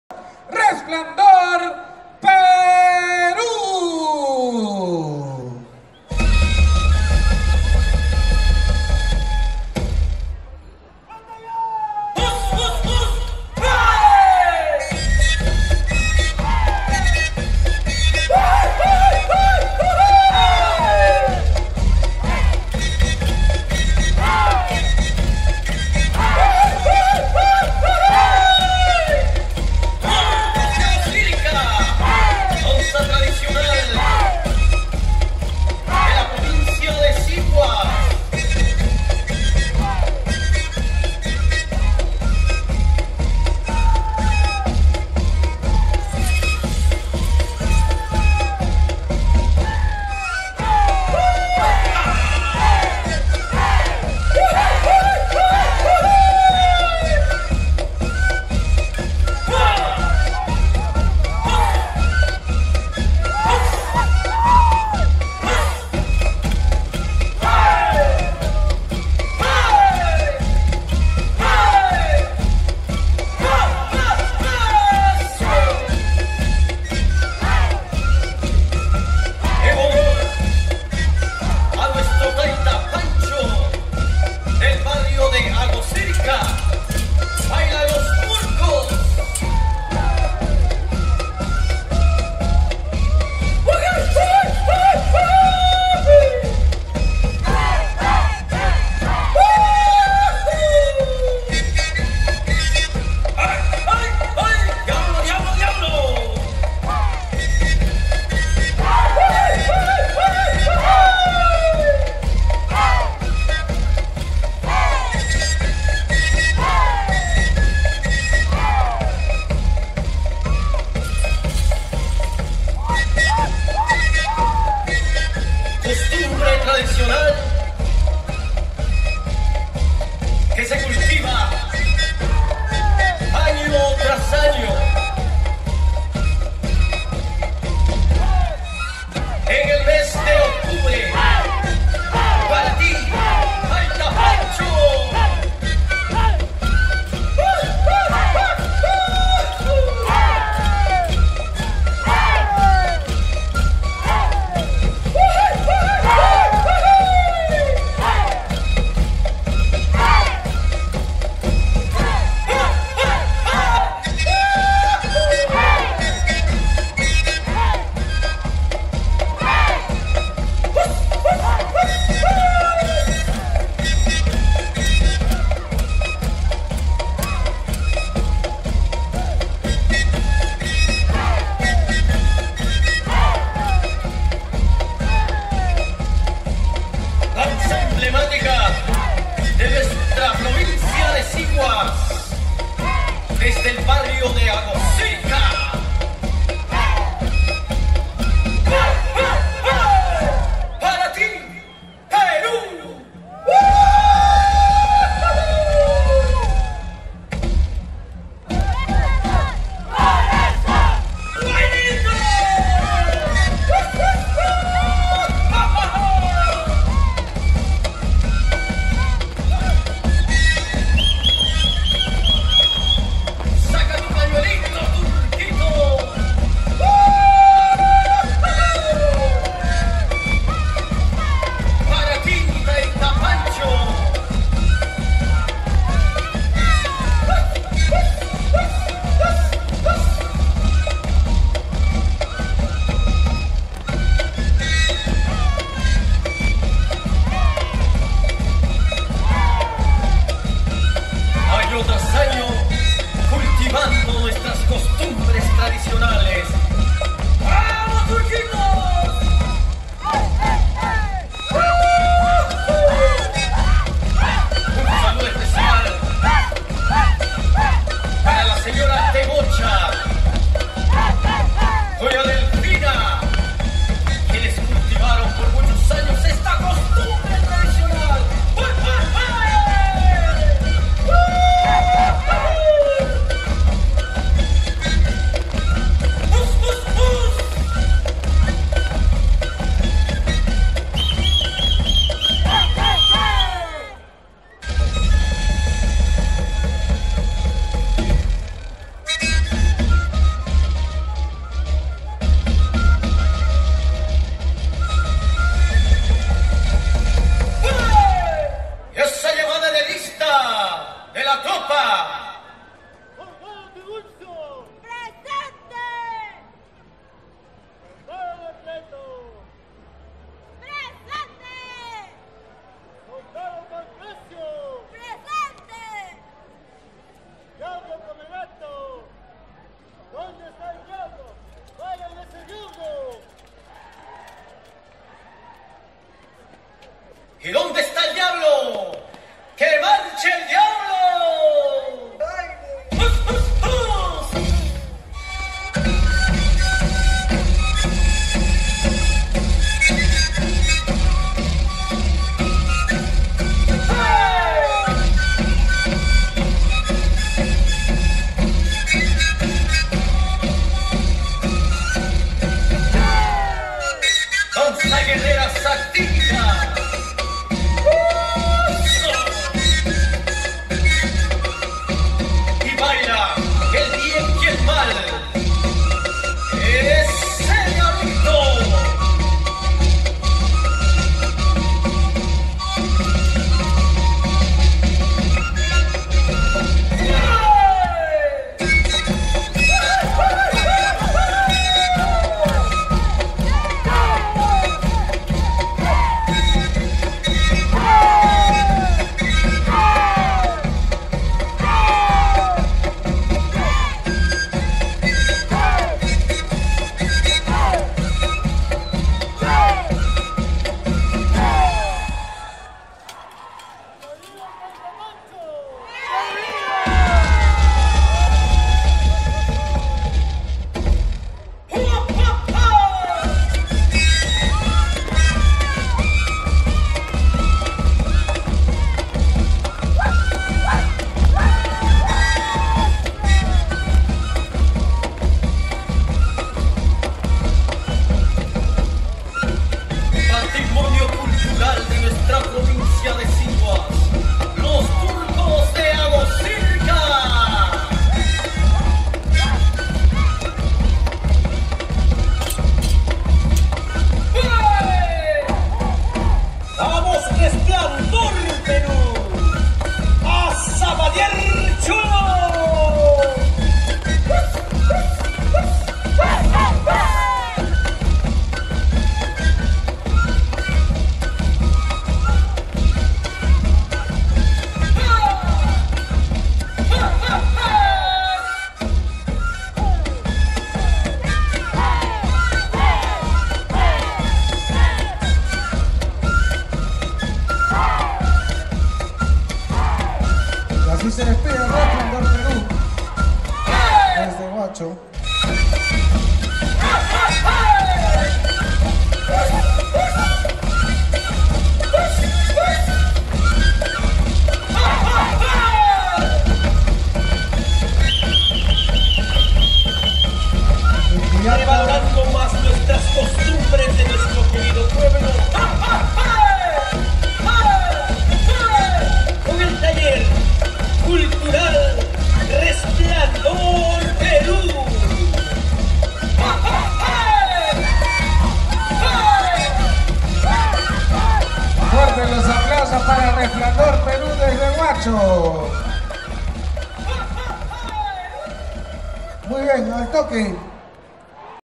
Turcos de Agoshilca - Resplandor Peru (Qory Wayna 2017 - Wayna Tupay)
Concurso nacional de danzas y estampas
"Qory Wayna 2017"
Organizado por  "CIDF Wayna Tupay"
Lima - SJM (Valle Sharon)
20/08/2017